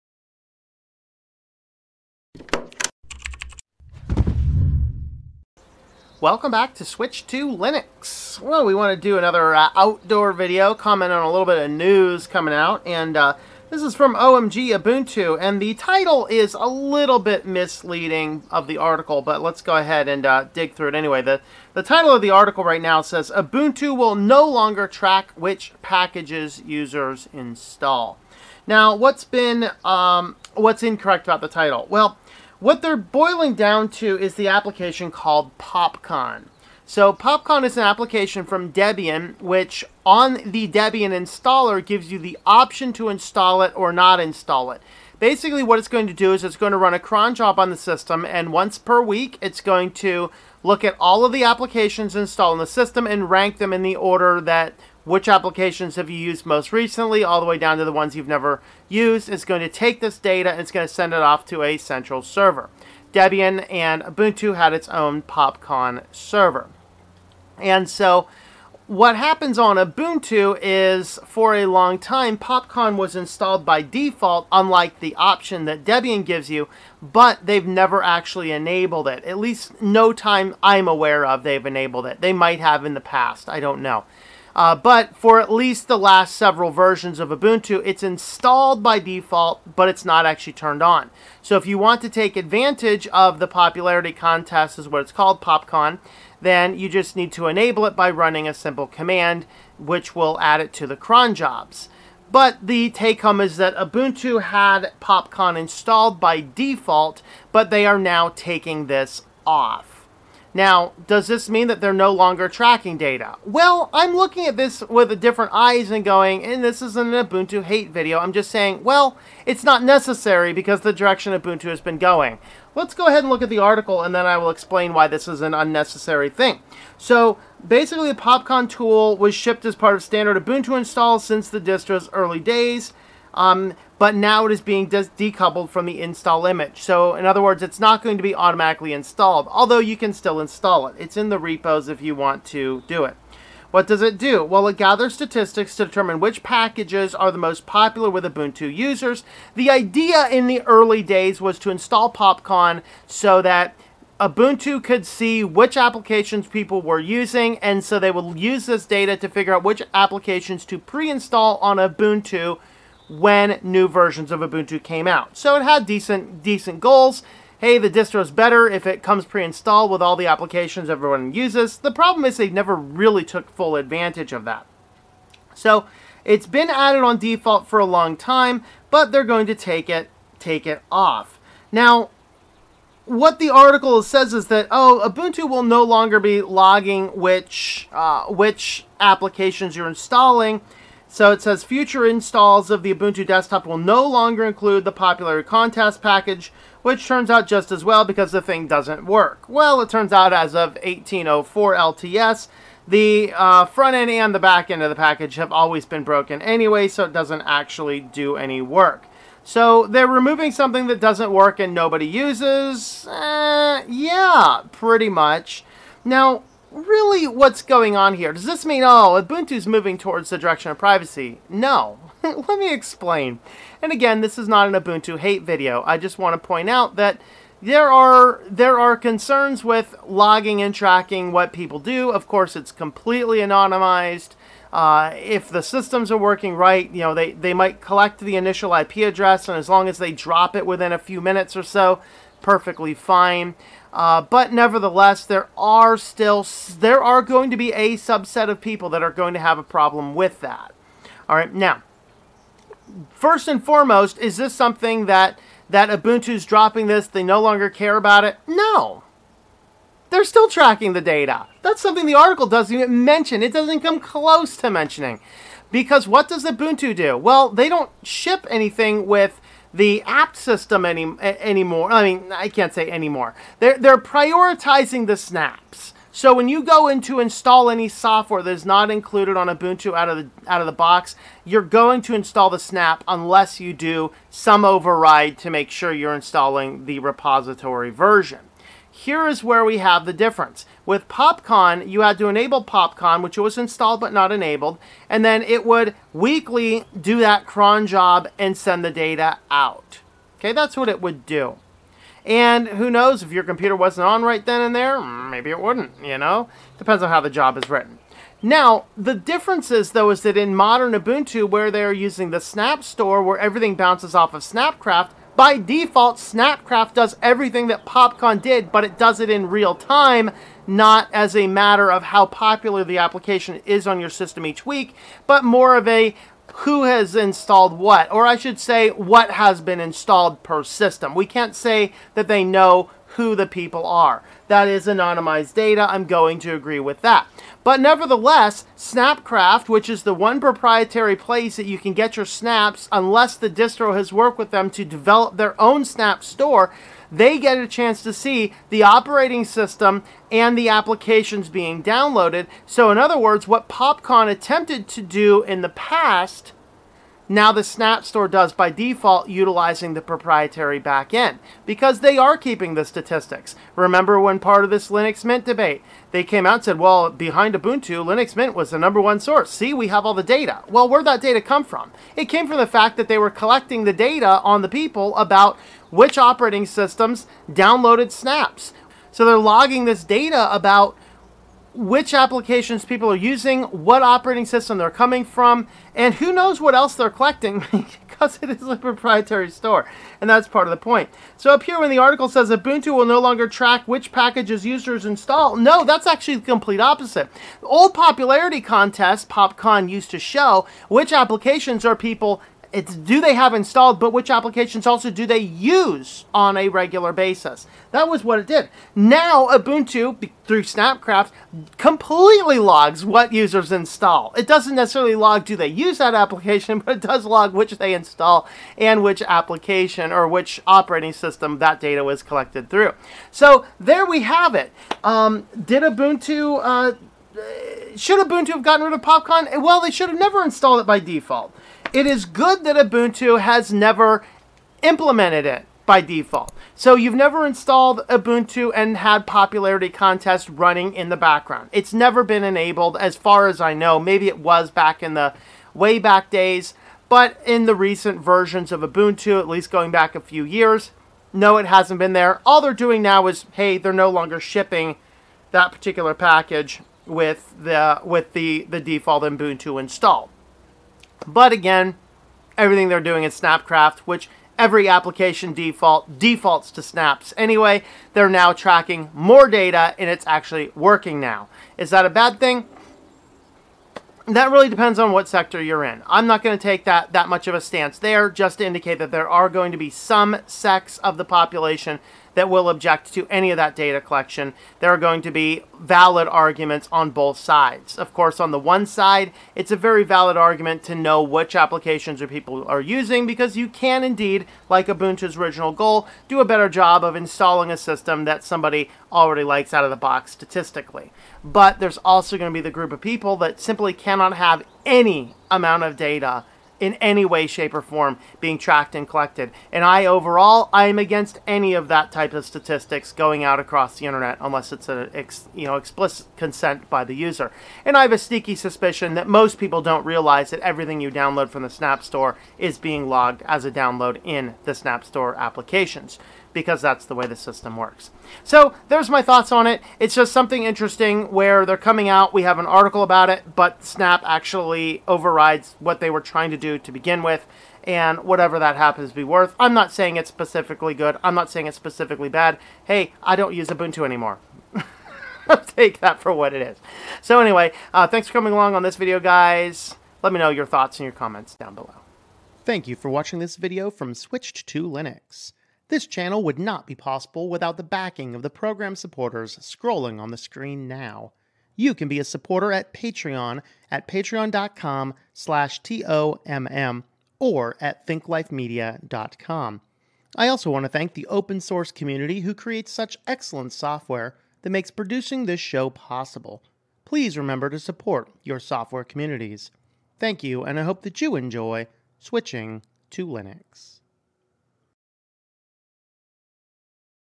Ubuntu Removes Popcon...They Are Still Tracking App Installs
Ubuntu will be removing PopCon and articles suggest that Ubuntu will no longer be tracking package installs...but actually, they are still tracking installs. Let's talk about it.

Support Switched to Linux!
🐸 Gab: @switchedtolinux
💡 Minds: @switchedtolinux